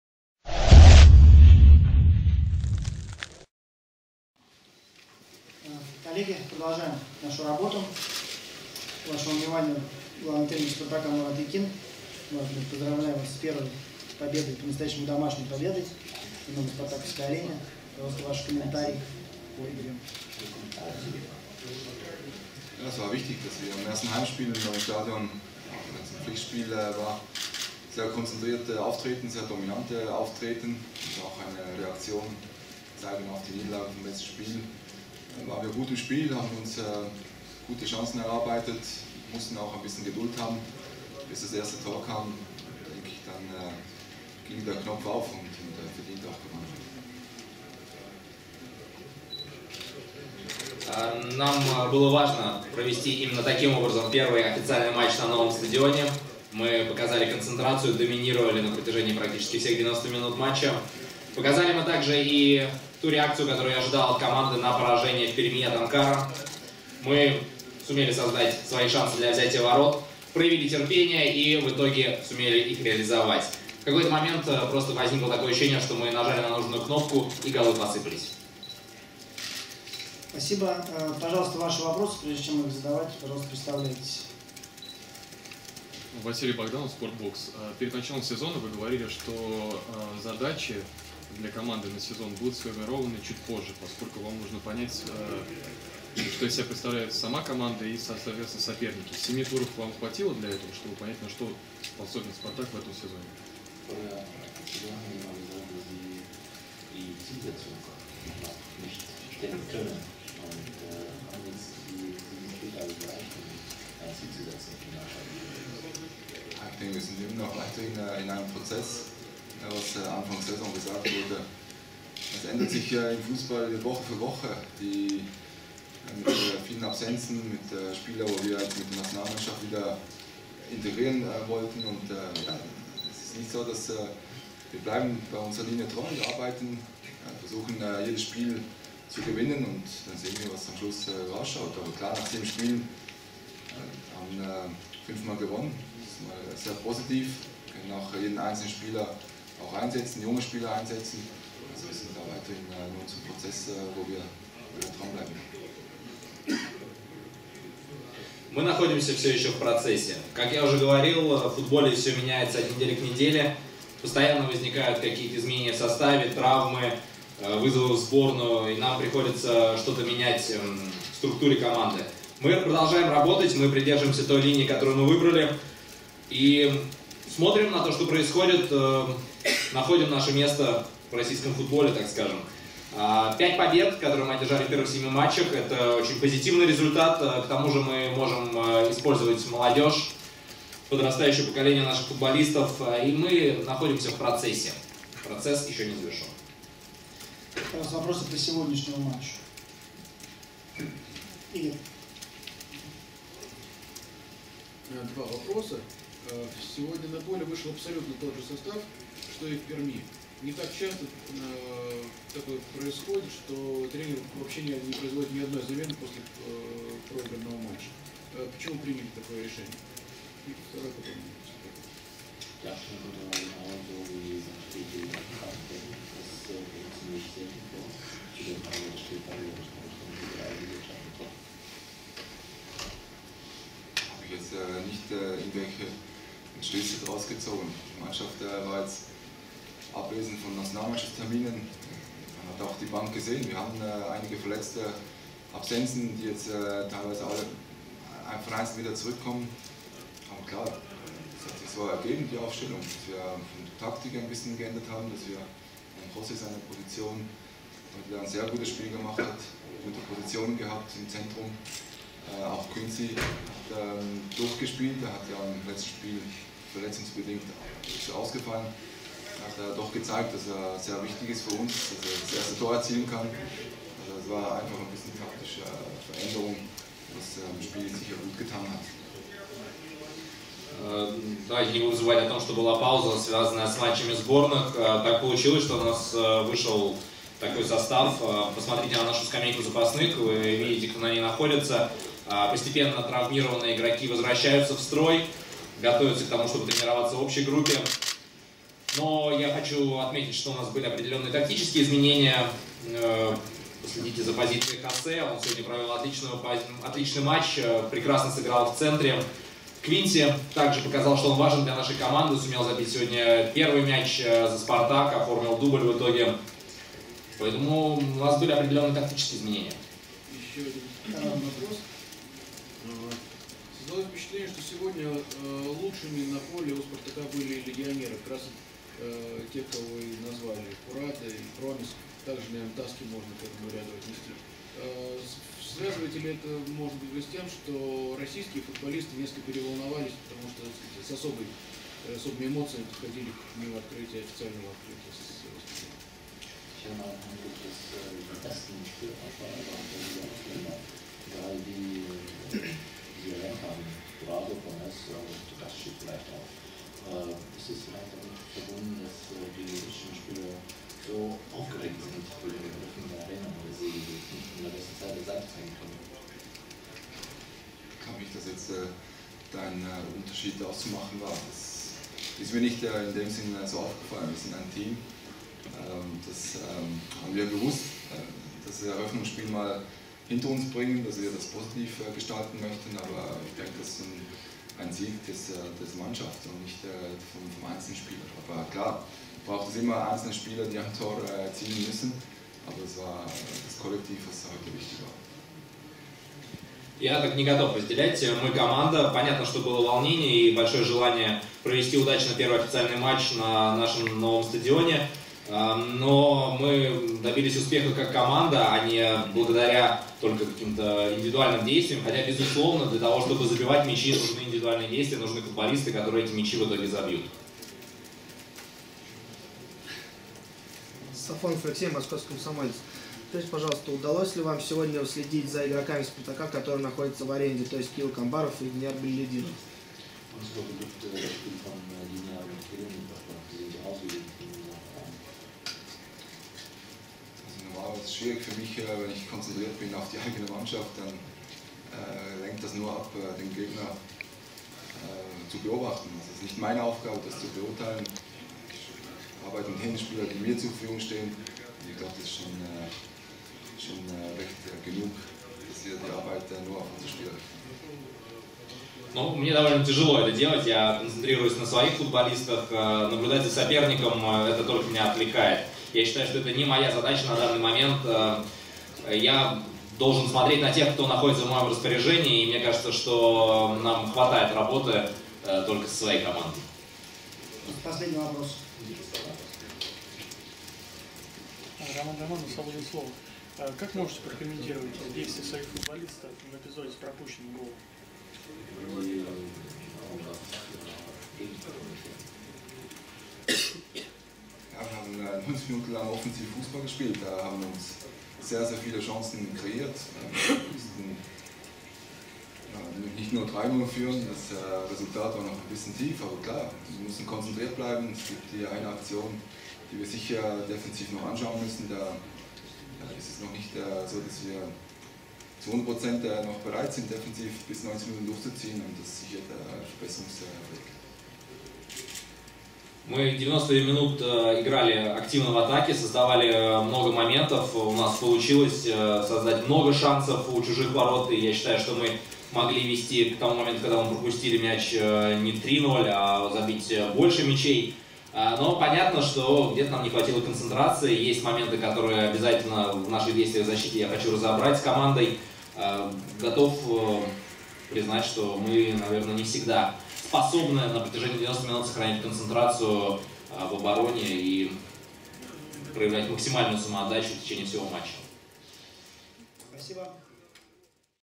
Мурат Якин о первой победе "Спартака" на новом стадионе! (HD) Fratria.ru
Послематчевая пресс-конференция Мурата Якина.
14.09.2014. «Спартак» — «Торпедо» — 3:1 (1:0)
Голы: 33´ Промес — 1:0; 70´ Промес — 2:0; 73´ Хурадо — 3:0; 89´ Виера — 3:1.